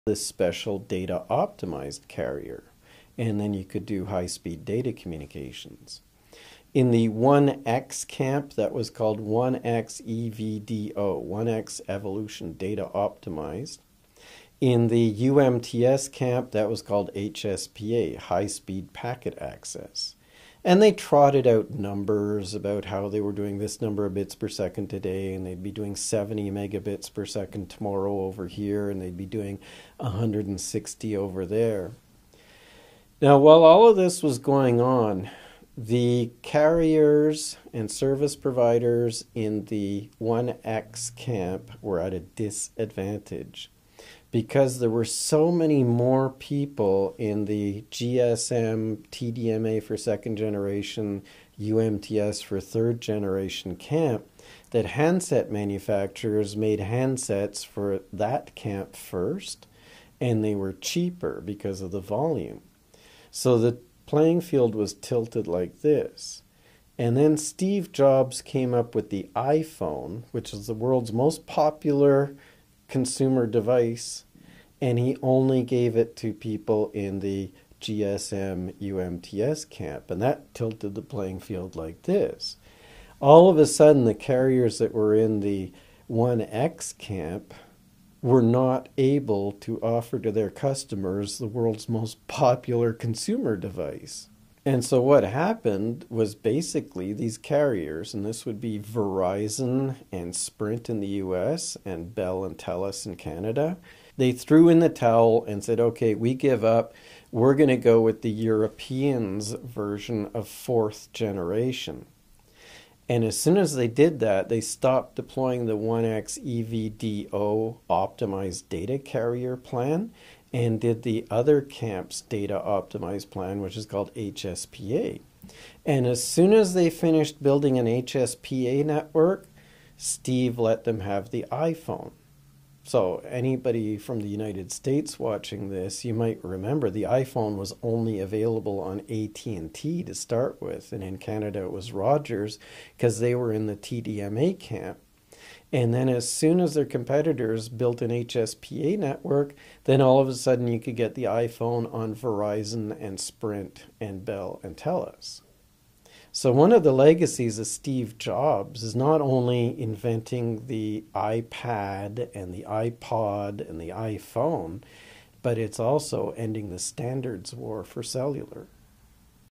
How Steve Jobs ended the cellular standards war with his iphone
Eric Coll relating how Steve Jobs ended the GSM-TDMA-CDMA-1X-UMTS cellular standards wars with his iPhone.

Before 4G, carriers had a choice of different, incompatible spectrum-sharing technologies.

The carriers formed into two camps:
- the camp that used 2G CDMA and 1X (Verizon, Sprint, Bell, Telus and others) and
- the camp that used 2G TDMA/GSM and UMTS (AT&T Wireless, Rogers, European carriers and most others).

Steve invented the world's most popular consumer device... and only let carriers in the GSM-TDMA camp have it.

Verizon, Sprint, Bell and the others threw in the towel to get the iPhone.  They agreed to go with the GSM-TDMA camp's plan for 4G, called LTE, and at the same time the GSM-TDMA camp's standard for 3G data, called HSPA.

The result was a worldwide single standard for cellular, starting with the fourth generation, thanks to Steve.

For the latest updates on Wireless see Course 2206 Wireless Communications